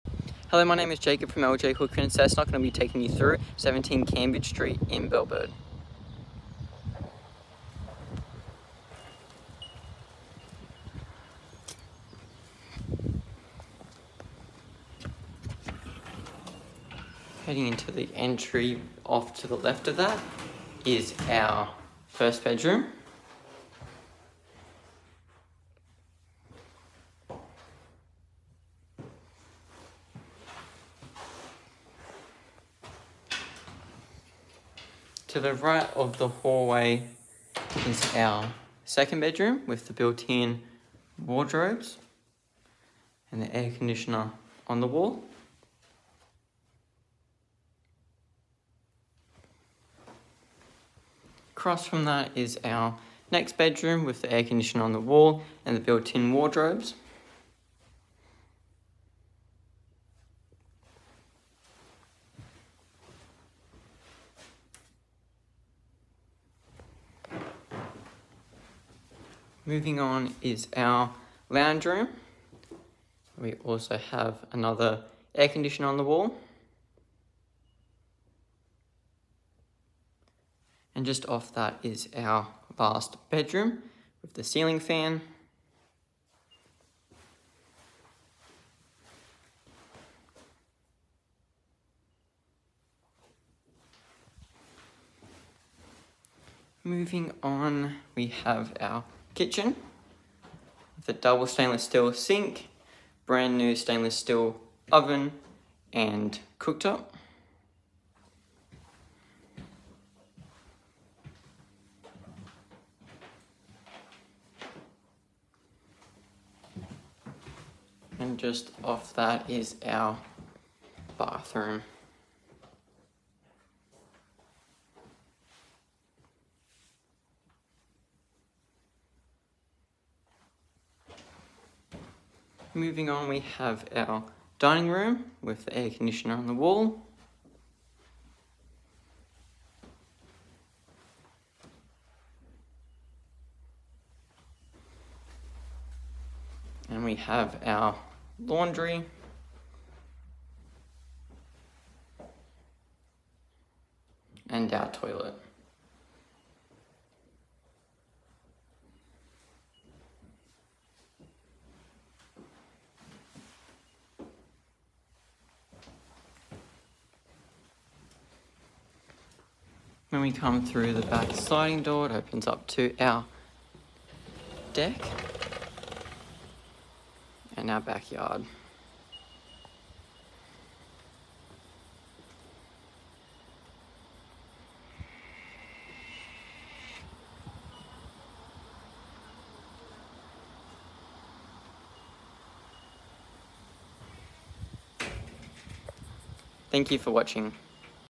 LJ Hooker Cessnock real estate agents present: 17 Cambage st Bellbird
Property management walk through January 2024